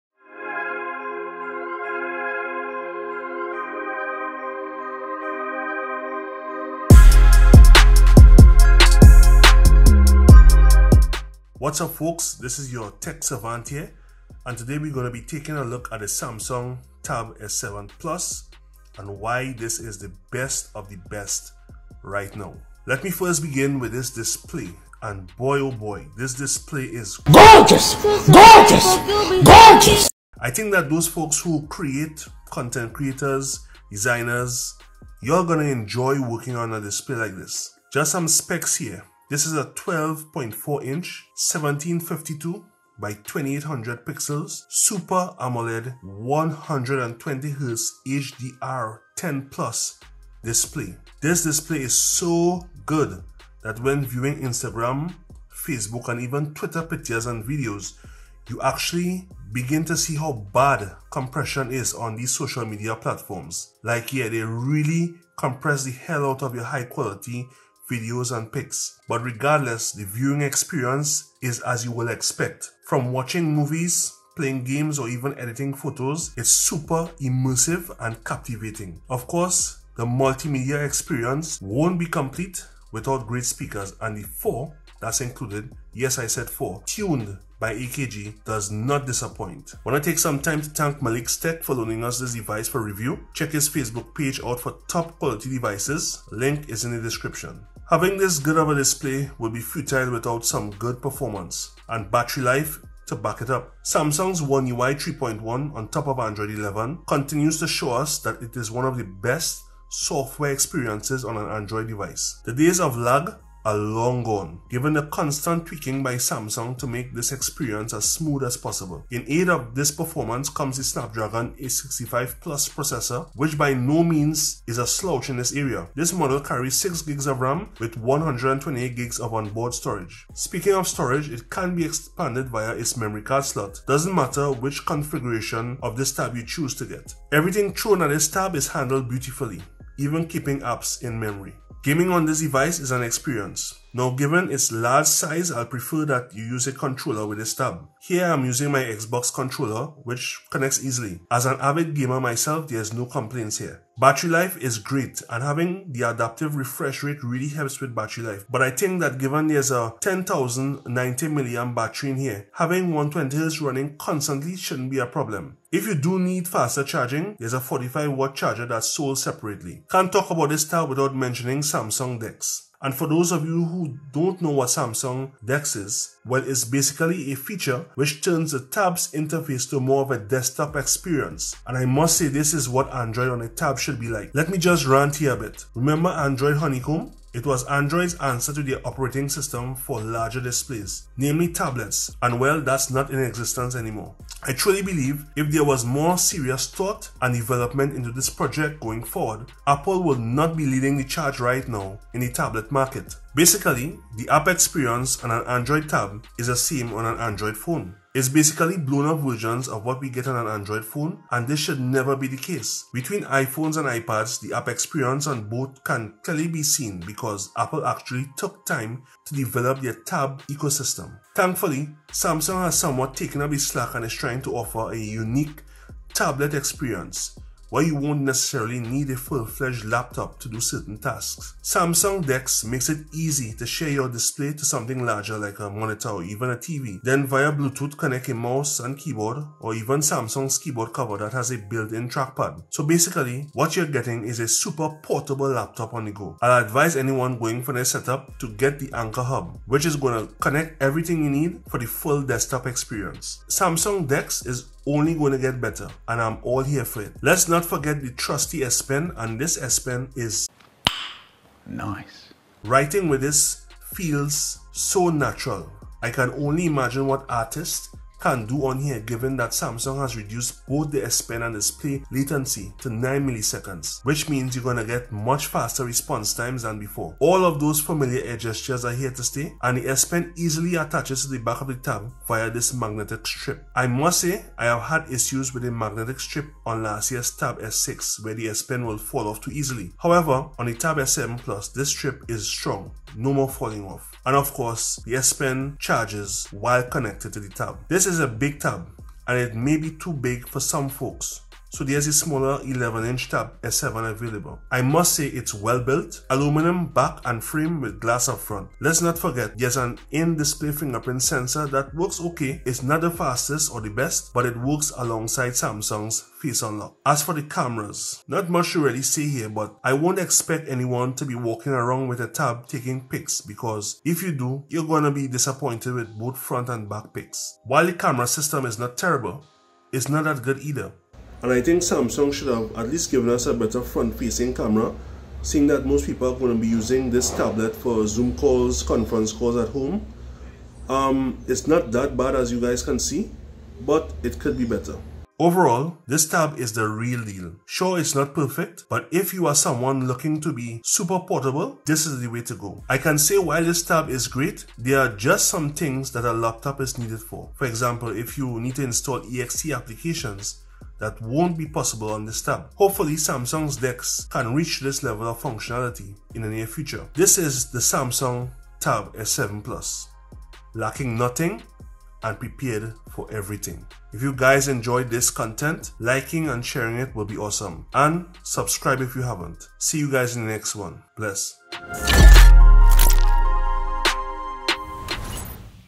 The Best Android Tablet to buy! - Samsung Tab S7 Plus Review
Ladies and Gentlemen look no further...The Samsung Tab S7+ is in my opinion THE BEST android tablet to get! Wana learn more join me as i take a dive into what makes this tab stand out above the rest!

Shoutout to Maleeq's Tech for loaning me this device for review! check out his page!

This entire video was shot on The Google Pixel 4XL